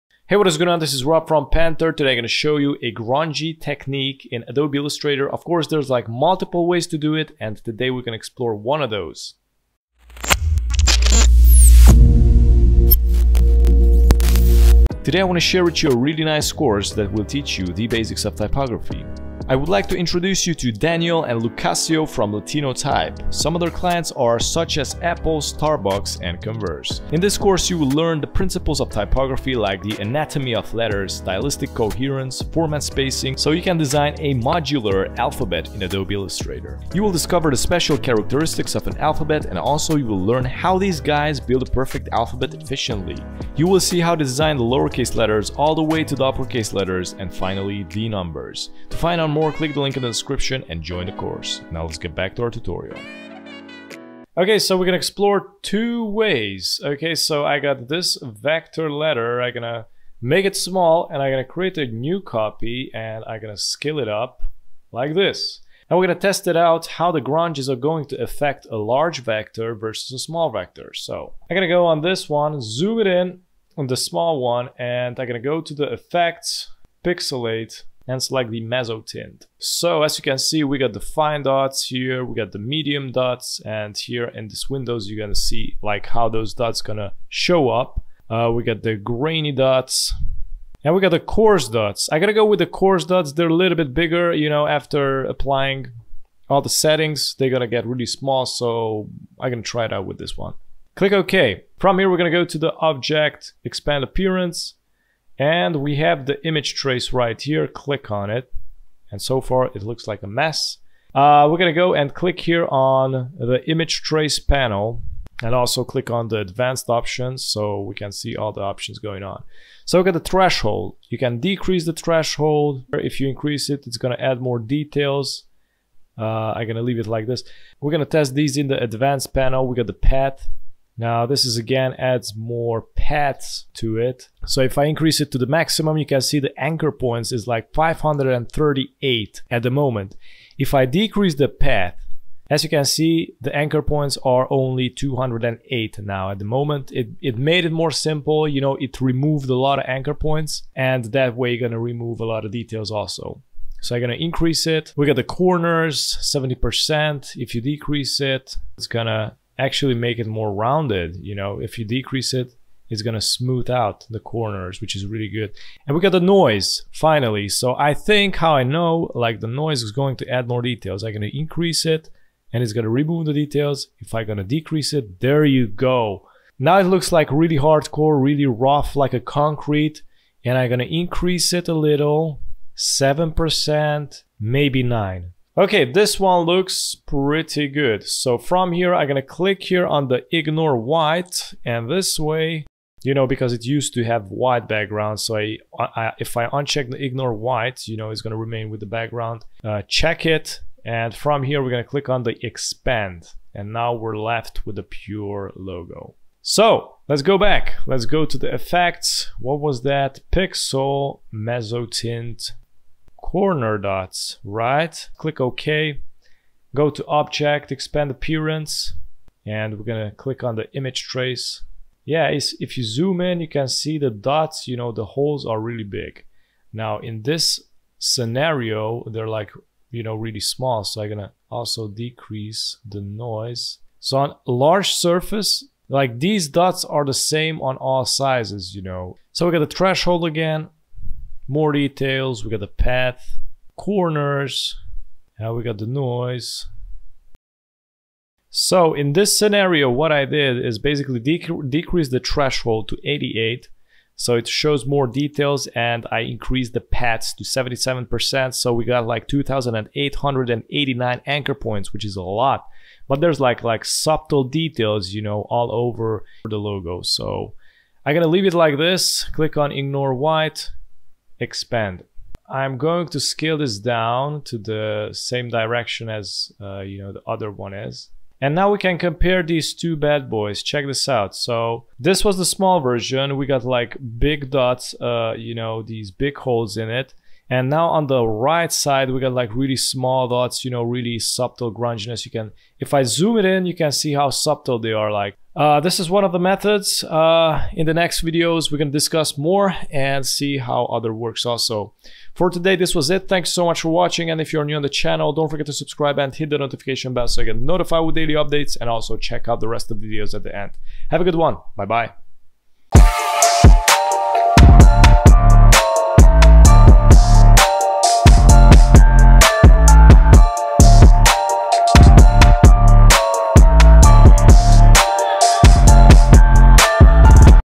How to Add Grunge in Illustrator
Today we gonna discover on of the directions how to add grunge effect in Adobe Illustrator. There are multiple ways to do it and I will show the rest in feature videos, but take a peak at this method first and enjoy!

PANTER social